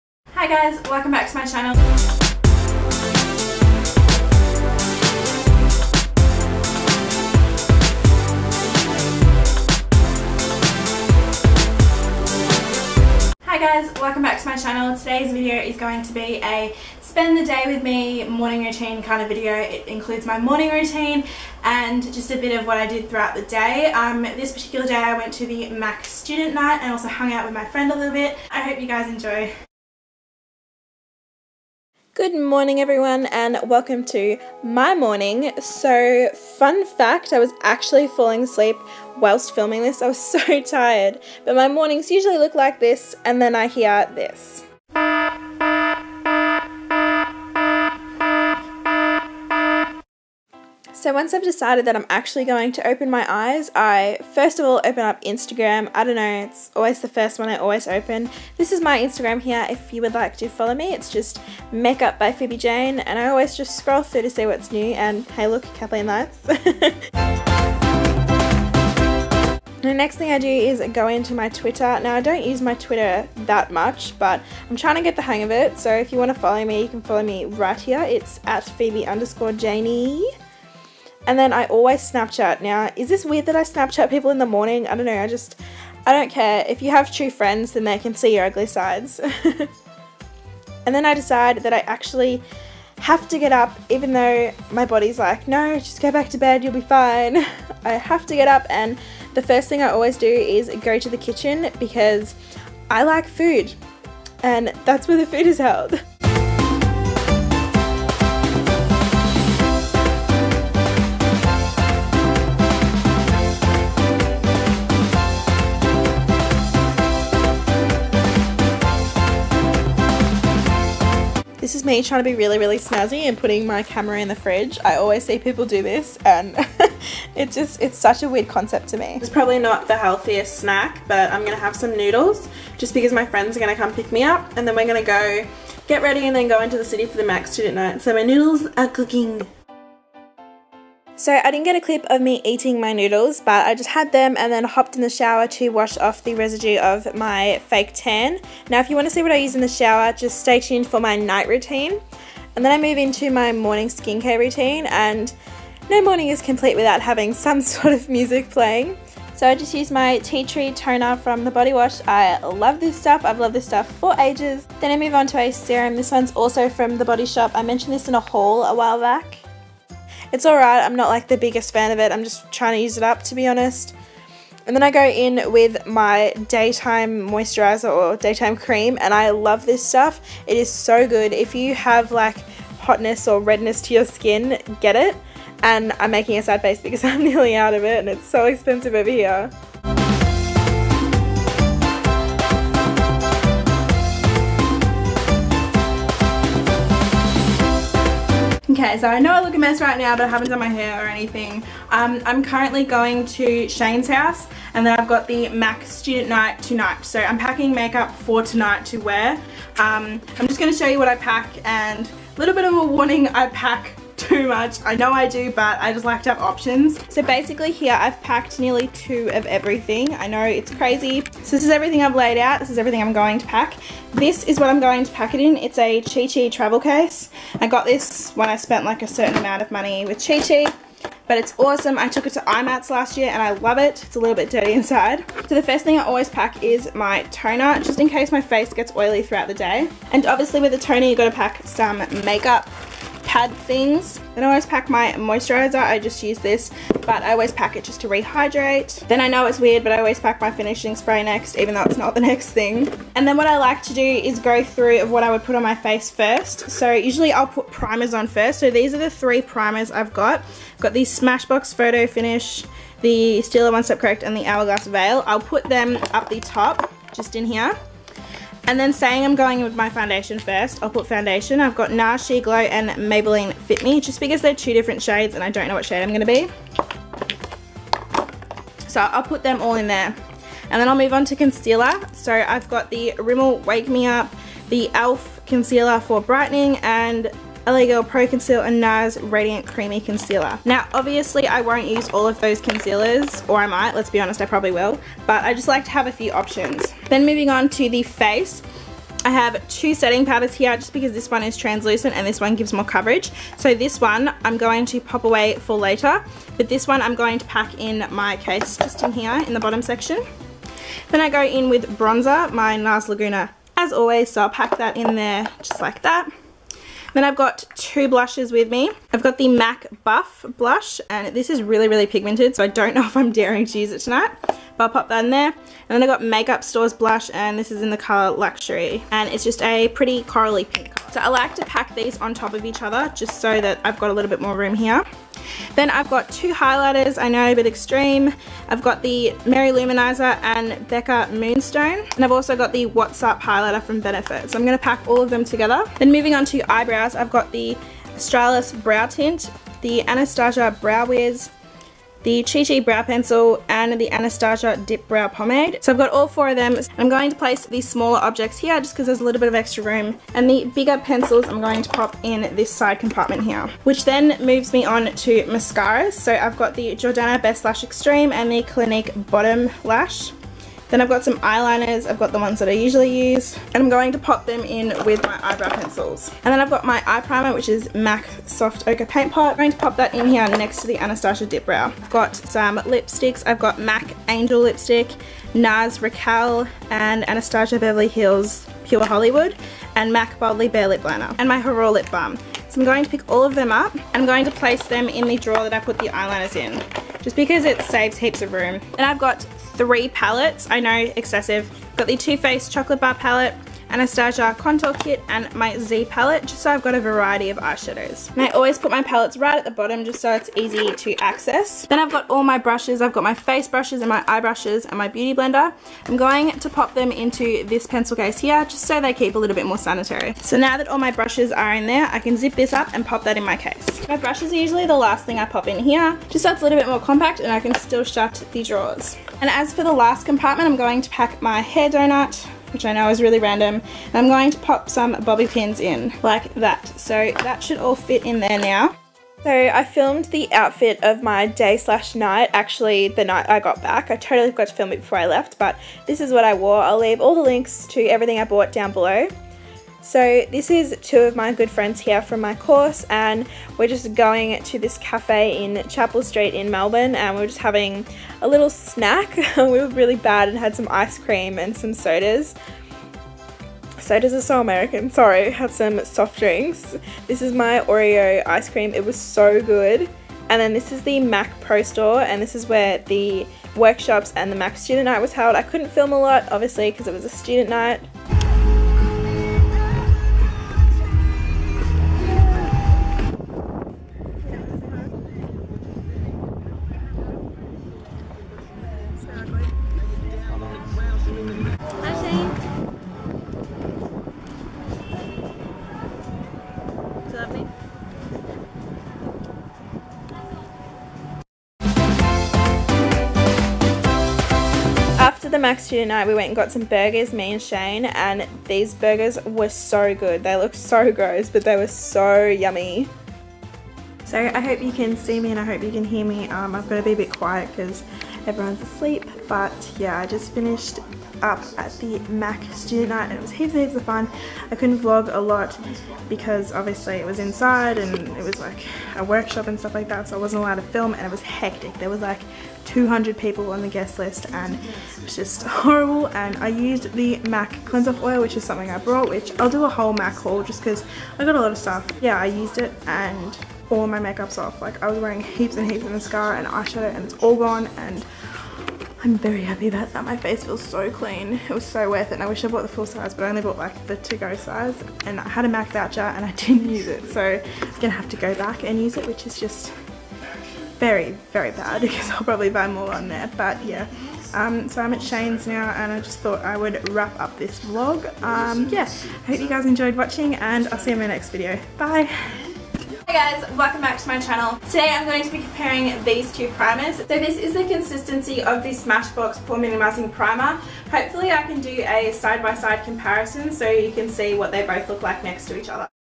Spend The Day With Me | Morning Routine | MAC Pro VIP Night ♡
Hey gorgeous, I hope you're having a wonderful day.This is a kind of different video. In this video I included my morning routine, my outfit, my skincare, what I pack to 'travel' and a bit of a MAC student pro night. Which means of course I filmed a MAC haul :p will be up soon!

Outfit:
- Top from boutique in shopping centre
- Skirt from Cocolatte
- Shoes from good old K-Mart

I'm obsessed with all things beauty, so come hang out and let's help feed each others beauty addictions! Haha
I upload roughly every 4 - 5 days! xx